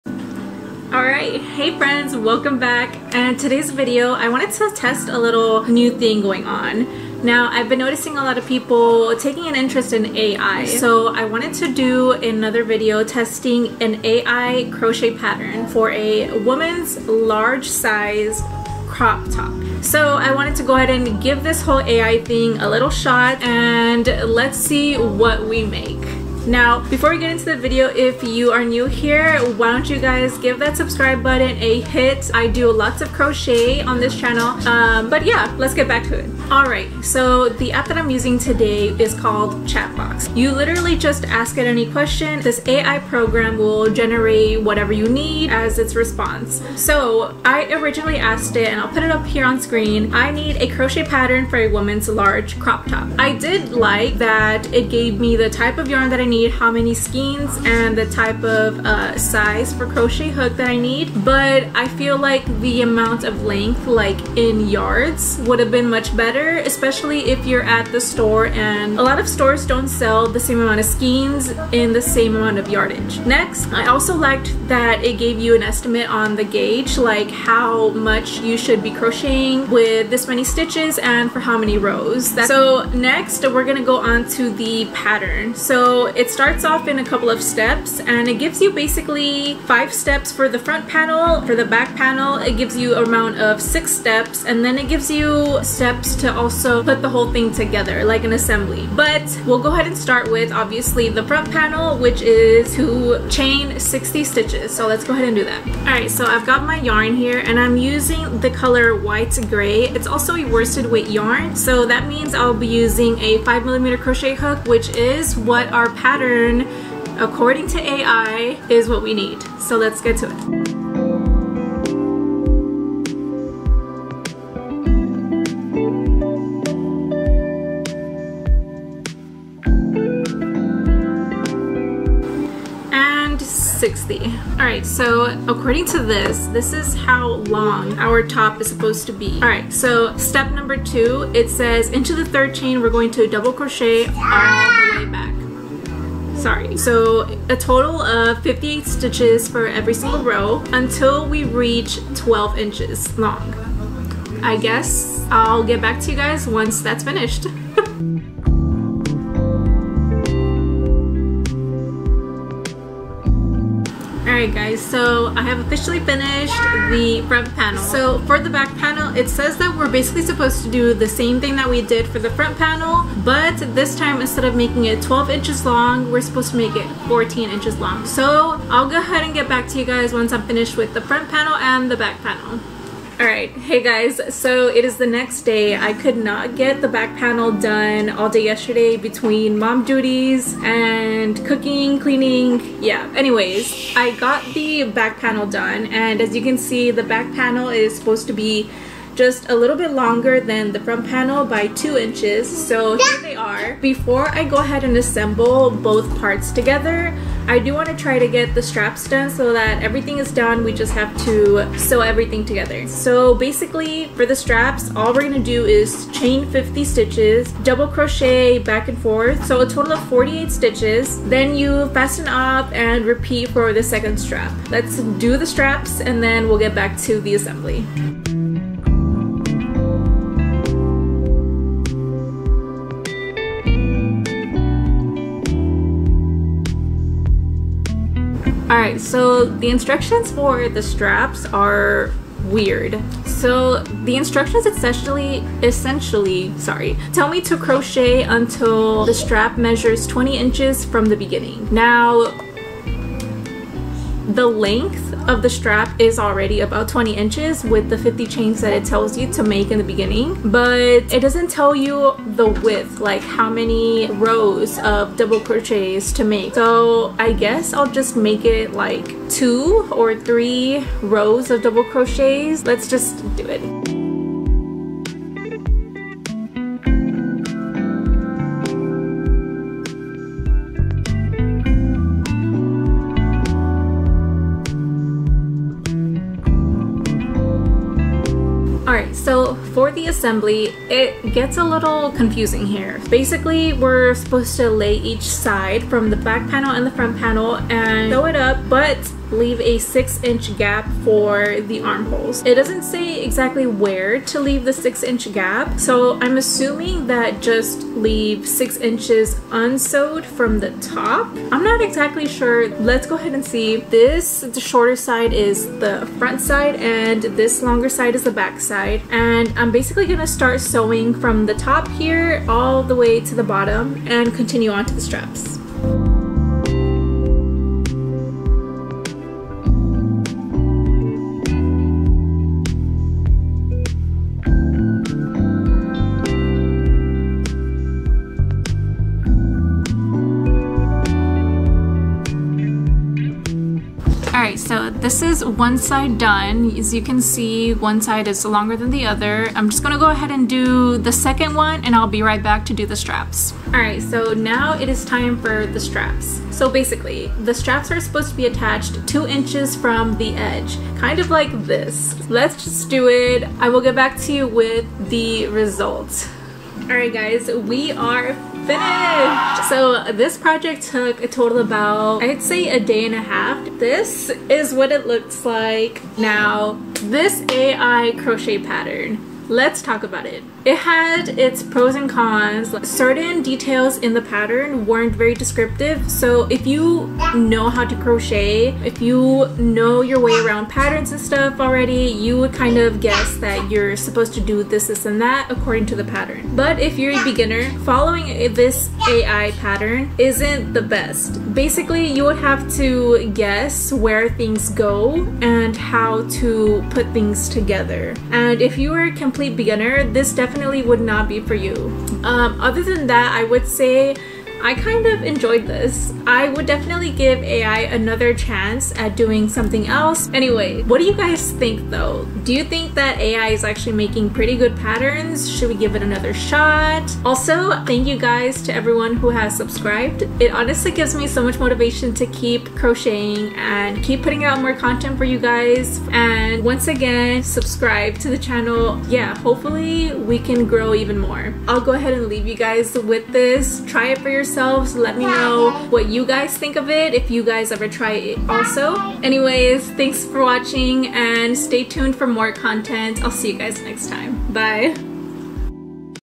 Testing AI Generated Crochet Pattern, SUCCESS!? | ItsRosyC
Hey friends!!

Today I am testing crochet patterns made by AI!
This pattern is for a Woman's large size crop top, I used a 5 mm crochet hook and worsted weight yarn about 7 skeins.
The pattern is on screen so feel free to pause If you wish to follow along and make yourself one too!